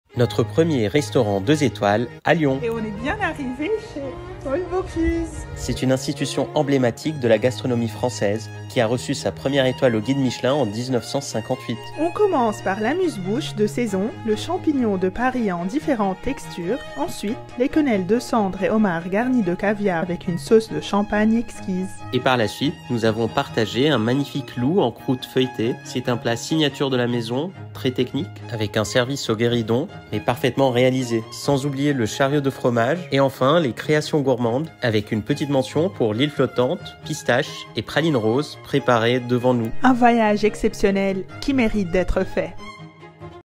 Paul Bocuse : 2 étoiles au Guide Michelin ⭐️⭐️
50 ans d’étoiles✨

✍️ Nous avons déjeuné dans le restaurant doublement étoilé de Paul Bocuse. Nous avons pris le menu au fil des saisons et c’est un sans faute ! Les plats sont tous excellents et particulièrement généreux.
De l’accueil au départ le personnel est au petit soin. Merci au chef et à toute l’équipe.

📍 40 Rue de la Plage, 69660 Collonges-au-Mont-d’Or @paul_bocuse_le_restaurant

💰 Menu Au fil des saisons 220€ par personne

📲 Follow nous @lefoodtravel pour plus d’adresses 🚨